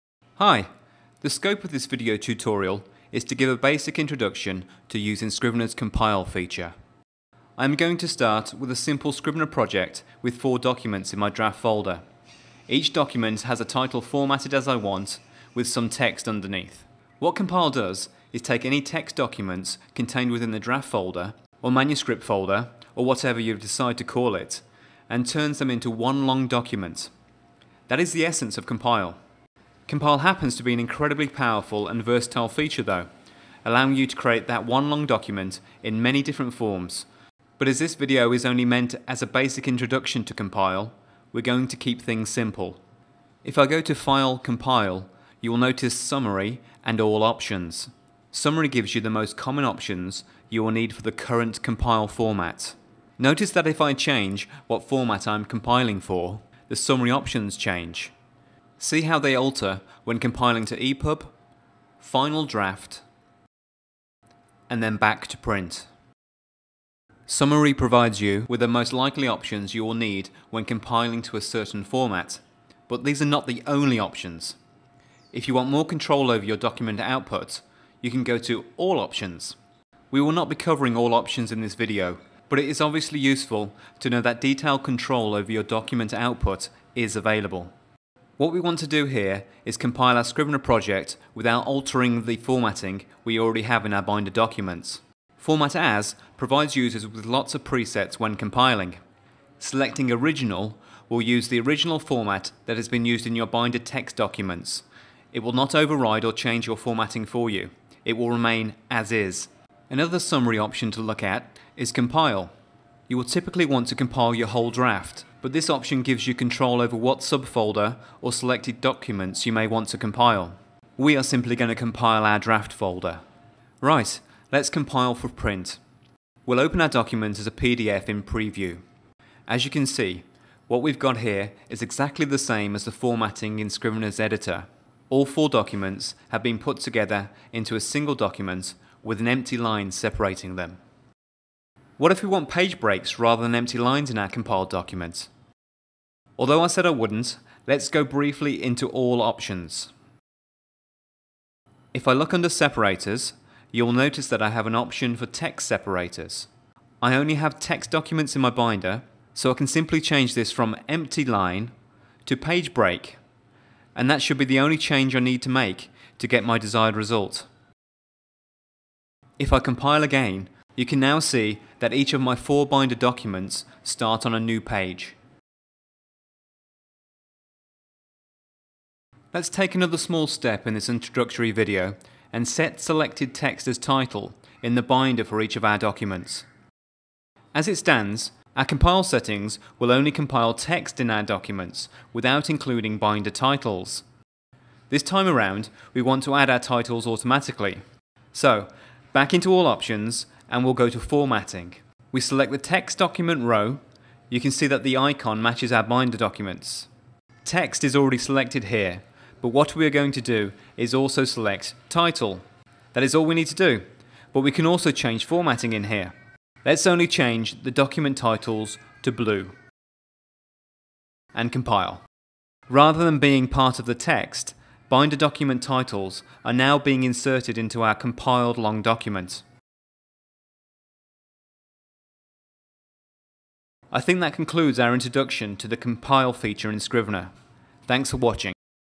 Scrivener 2 Basics - Introduction to Compile
Tutorial video introducing the basic features of compile in Scrivener.